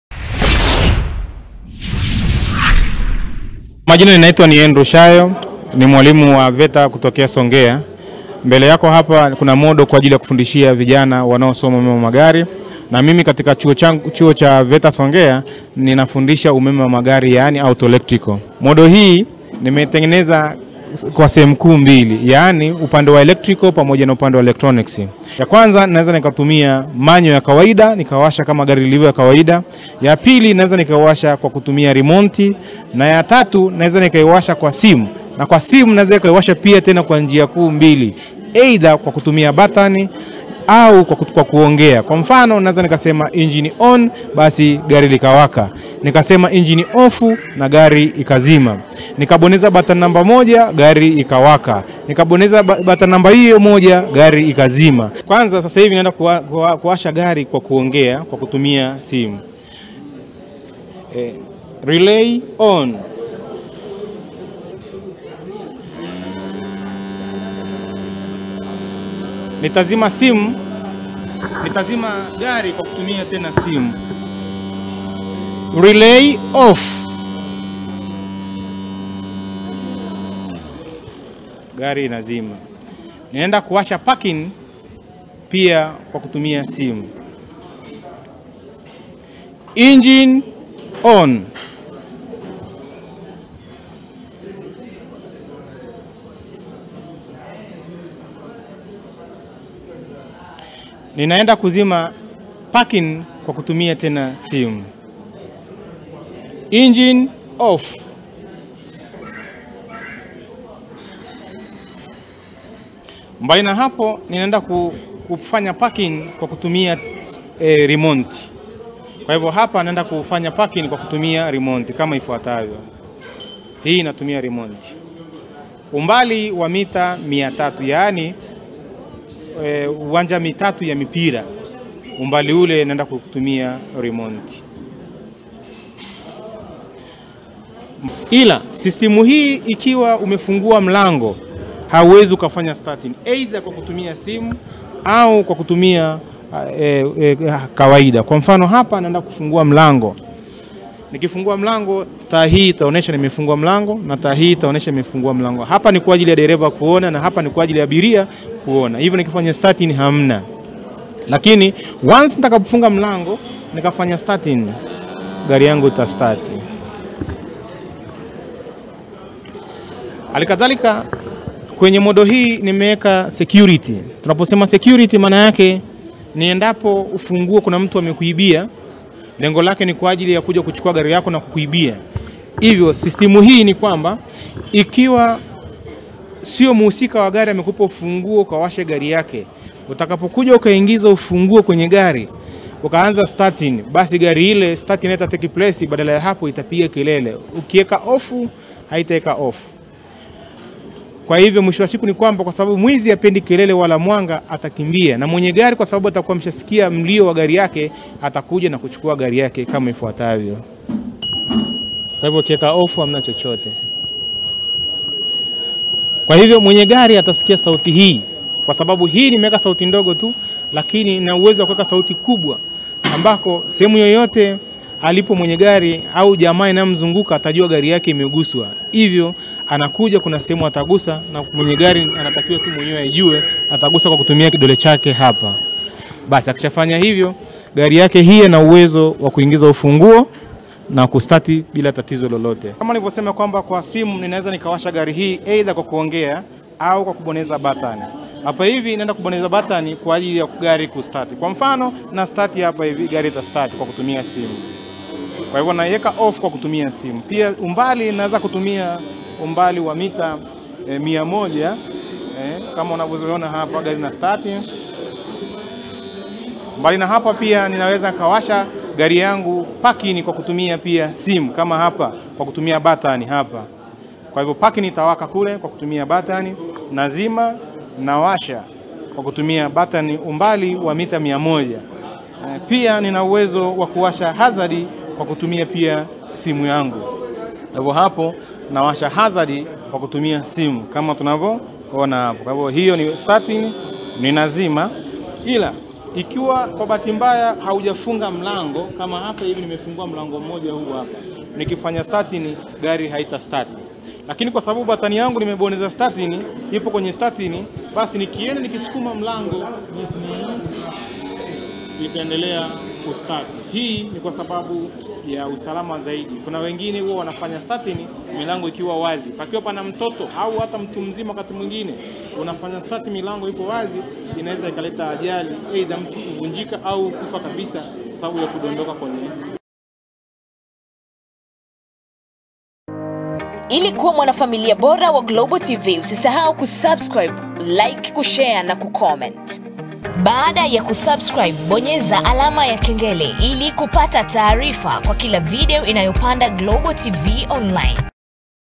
TEKNOLOJIA MPYA VETA: KUWASHA GARI KWA KUTUMIA SIMU, SAUTI YAKO INAWEZA KUFUNGUA MLANGO..
Mtaalamu wa magari kutoka chuo cha Veta, amebuni Teknologia mpya ya kuweza kuwasha gari kwa kutumia simu au kwa kuongea kupitia simu kisha gari kuweza kuwaka.

Teknologia hii ni tofauti na ile ya kuwasha gari kwa kutumia ufunguo, ambapo dereva umlazimu kuingia ndani ya gari, lakini teknologia hii unaweza kuwasha gari kwa kutumia simu kwa kuongea na kubonyesha simu ukiwa hata umbali wa mita 100.